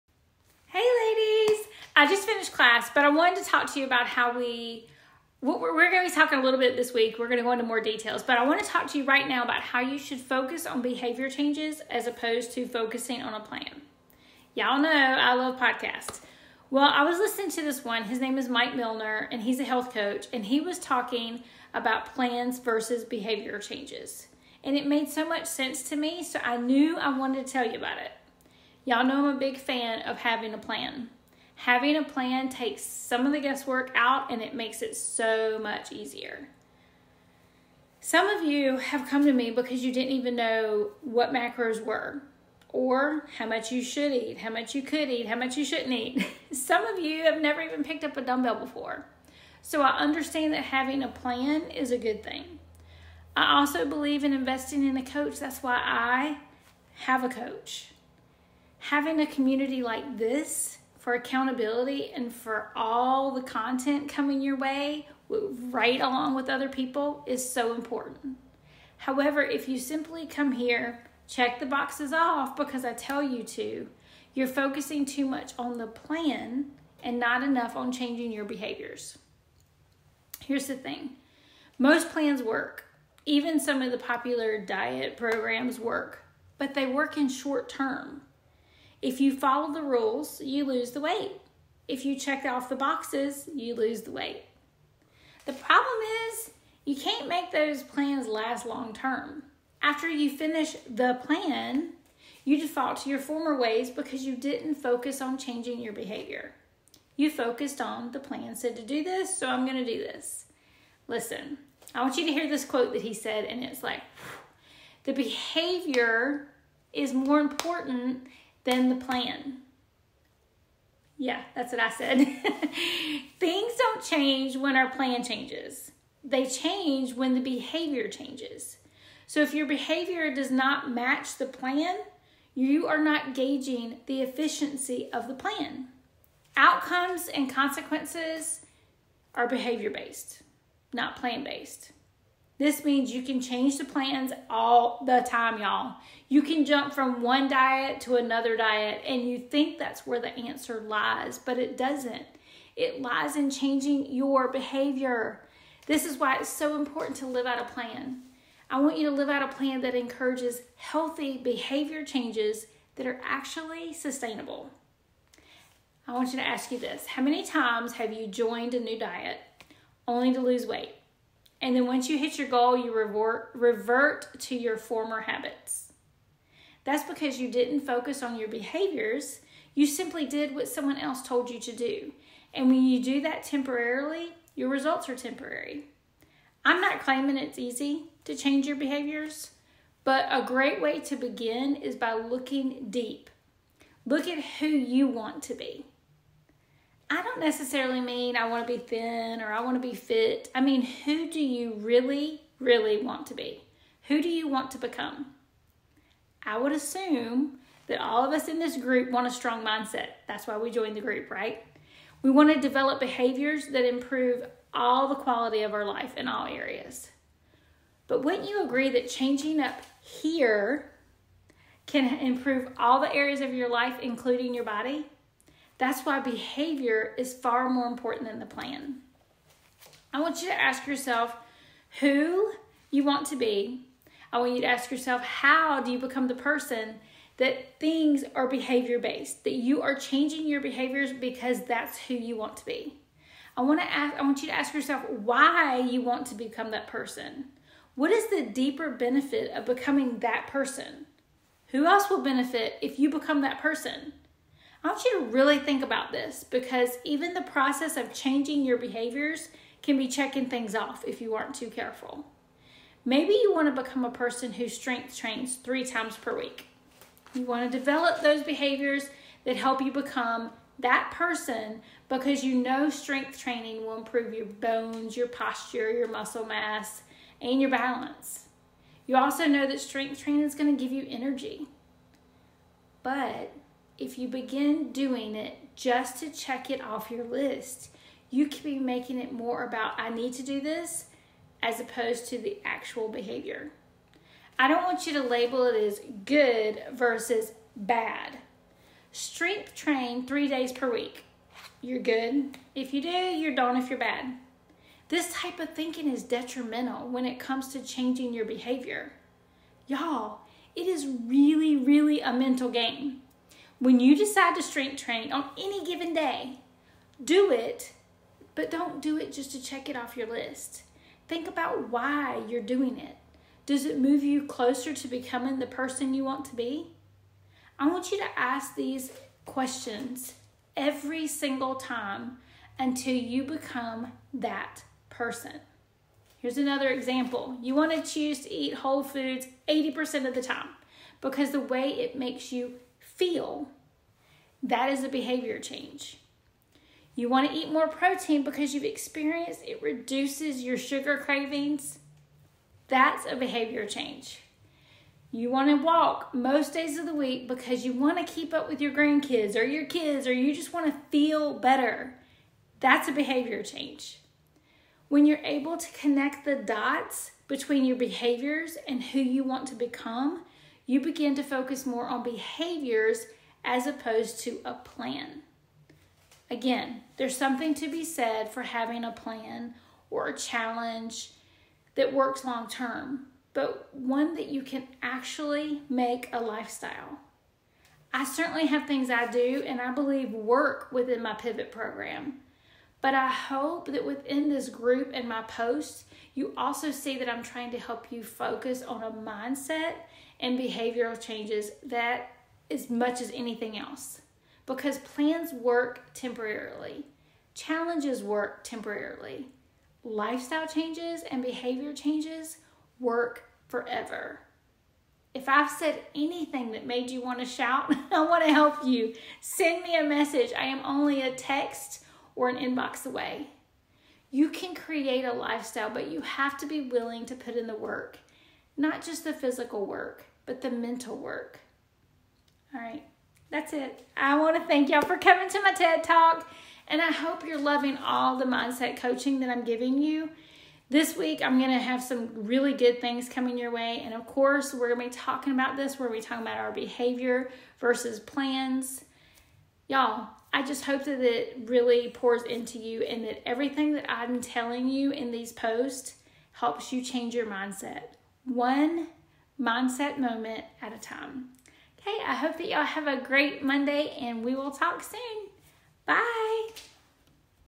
Mindset Minute - Behaviors VS plans
Are you chasing the perfect "plan". or "diet" to give you the body that you desire?

Listen to this Mindset Minute so you can discover what makes LONG TERM successful plans WORK!!